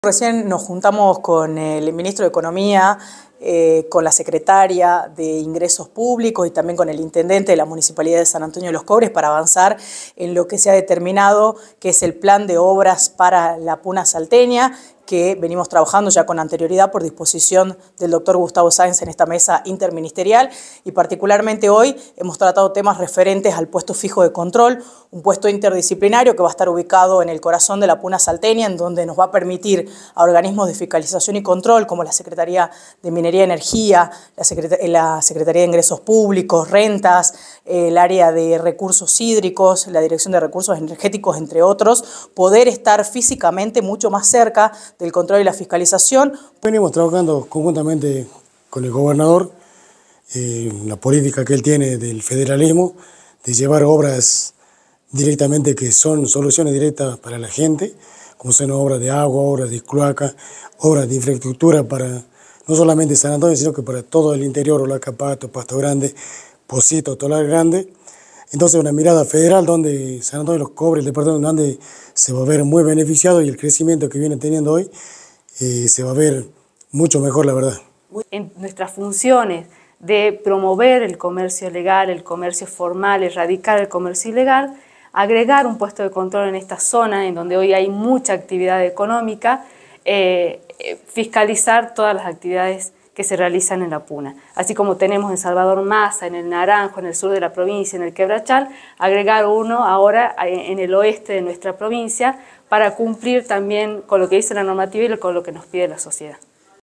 Avanza el proyecto de construcción de un puesto de control en la Puna salteña
Notas

Soledad Claros, secretaria de Ingresos Públicos
Romina Sassarini, secretaria de Minería y Energía
Alberto Carral, intendente de San Antonio de los Cobres